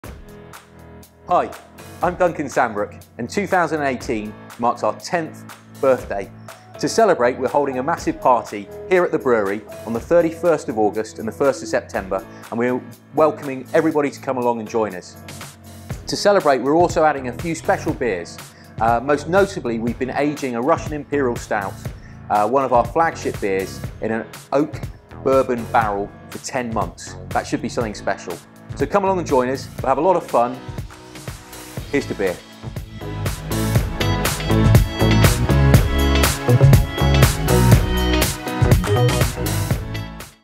Sambrook's BEER BY THE BREWERY!
2018 marks 10 years since we brewed that very first drop and we want to celebrate our birthday with everyone who's supported us in the last decade. But 1 party didn't sound like enough... so we're having 10!

For the past few years we've been running our ever-popular seasonal mini-festivals at the brewery. And because this is a special year for us, we wanted to up the ante a little bit.
So join us over a late summer weekend for a street party right here in Battersea! We'll have a stage for live music and DJs, loads of street food vendors and local artisanal goodies, there'll be free tours for you to see where the magic happens, and of course we'll have fresh craft beer galore all brewed just a matter of yards from where it'll hit your lips in the summer sun.

The Friday evening is an extra special affair where tickets include unlimited tasting of all our beers on offer that evening, PLUS a one-off tasting including: a last edition bottle of our No.5 Barley Wine brewed 5 years ago, and a 1/3 taster of our Russian Imperial Stout aged in a Bourbon beer barrel. We'll also be firing up the BBQ with some special treats which will be included free with all Friday VIP tickets. The ticket also includes entry only to the party on Saturday.
The Saturday is a family-friendly all-day street party designed for those wanting to swing by and enjoy some beers, food and music in the sun.

We'll update this page as we go with details of the line ups. For now, grab your tickets and wait for the good times to roll!
To stay up to date on all the news of our 10for10 parties, sign up to our newsletter or follow us on

* Please note, exact content of goody bag TBC and items may change between now and the event.